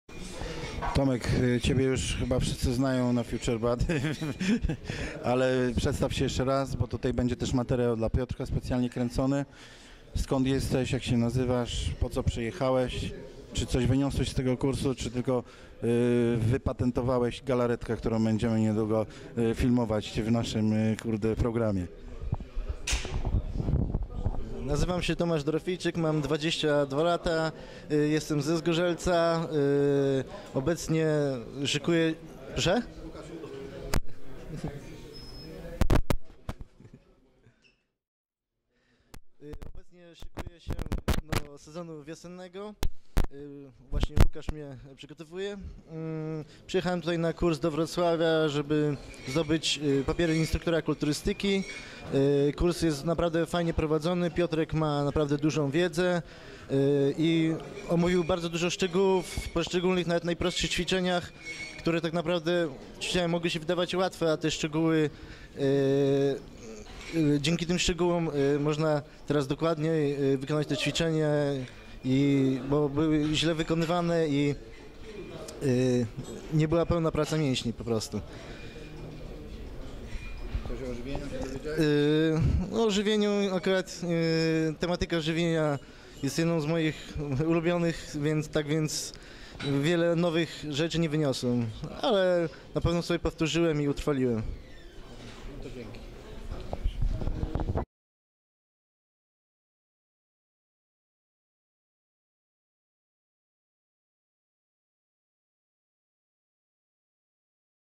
Kurs instruktora kulturystyki prowadzący Piotr Głuchowski FUTUREBODY - Tomasz Dorofiejczyk
Wywiad z Tomaszem Dorofiejczykiem (Zgorzelec) - uczestnikiem kursu na instruktora kulturystyki w Siłowni FUTUREBODY we Wrocławiu.